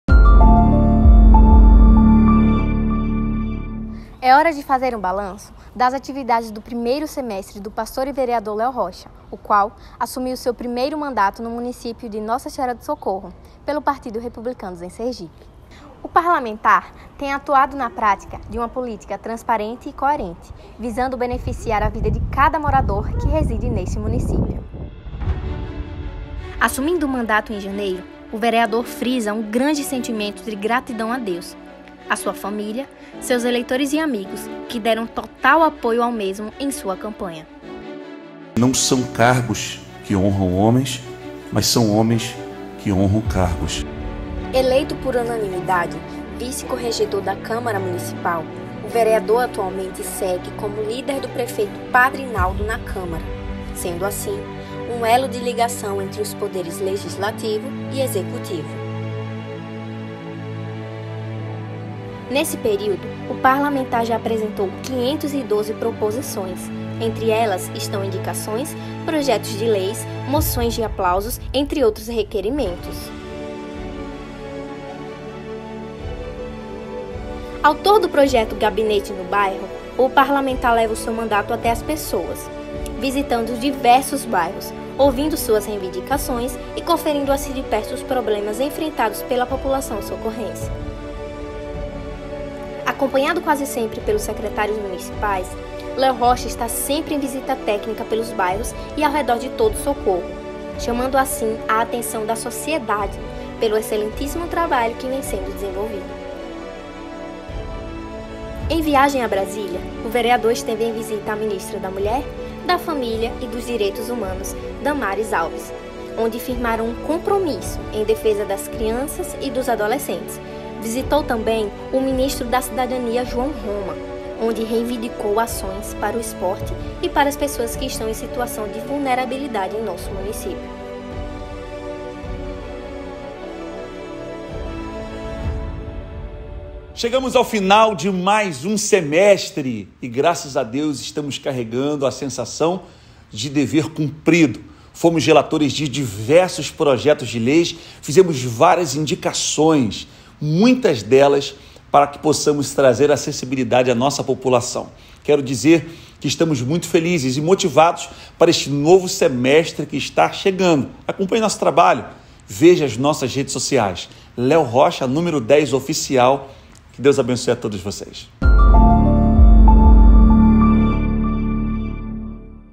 Balanço Parlamentar | Leo Rocha
Acompanhe nosso trabalho nesse primeiro trimestre de 2020, esse é o mandato do Pastor e Vereador Leo Rocha.